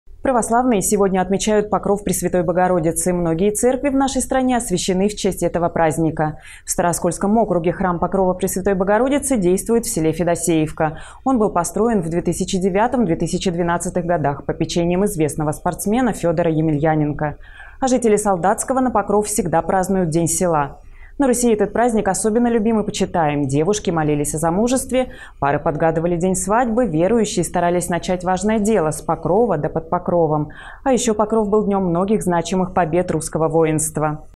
Православные отмечают Покров Пресвятой Богородицы
Многие церкви в нашей стране освящены в честь этого праздника. В Старооскольском округе храм Покрова Пресвятой Богородицы действует в селе Федосеевка, он был построен в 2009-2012 годах попечением известного спортсмена Фёдора Емельяненко. А жители Солдатского на Покров всегда празднуют День села. На Руси этот праздник особенно любим и почитаем: девушки молились о замужестве, пары подгадывали день свадьбы, верующие старались начать важное дело "с Покрова да под Покровом", а еще Покров был днем многих значимых побед русского воинства.